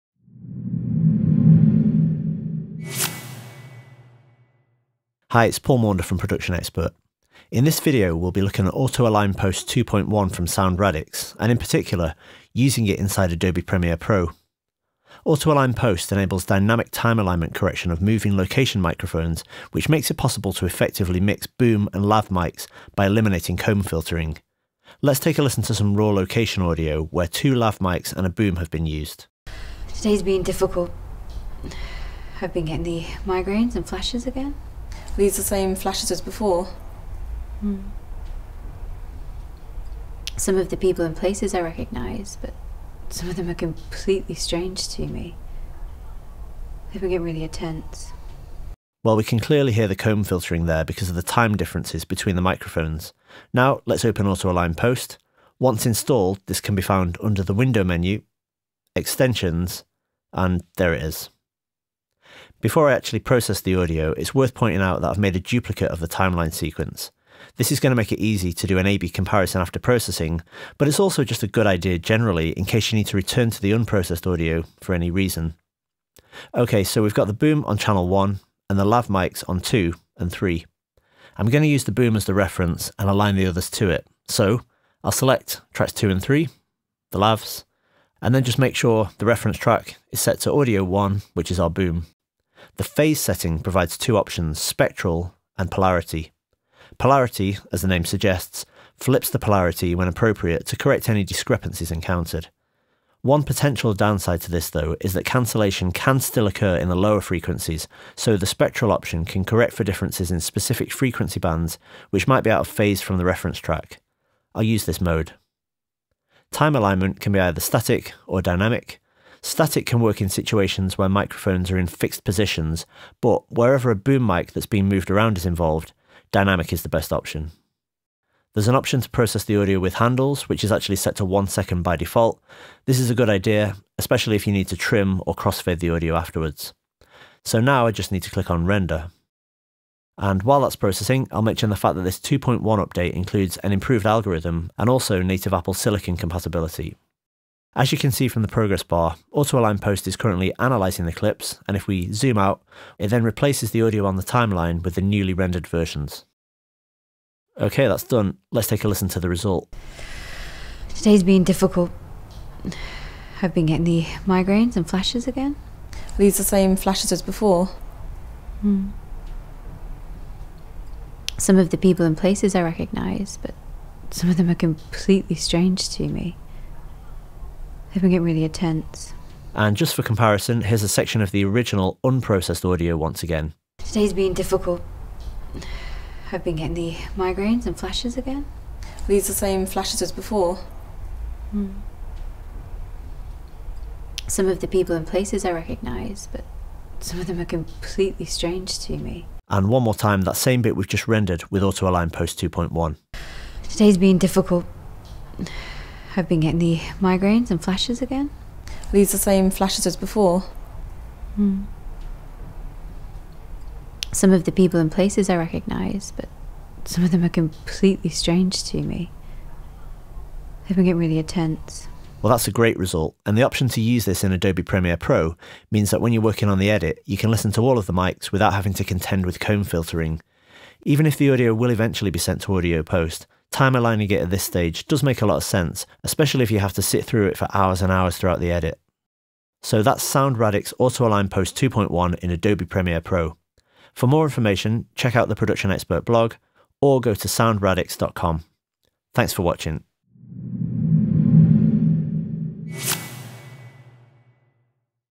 Using Sound Radix Auto Align Post 2 In Adobe Premiere
One of the headline stories in the recent Auto Align Post 2.1 update from Sound Radix was Adobe Premiere Pro compatibility. In this article and video, Paul Maunder puts it to the test with some raw location audio.

Auto Align Post 2 can be used to time align location recordings from multiple microphones directly within the timeline of a Premiere sequence, correcting the comb filtering issues caused by the timing differences between the microphones.